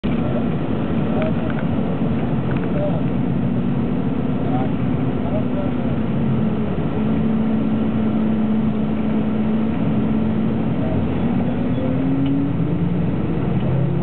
Bamako Market
Driving through the streets of Bamako, Mali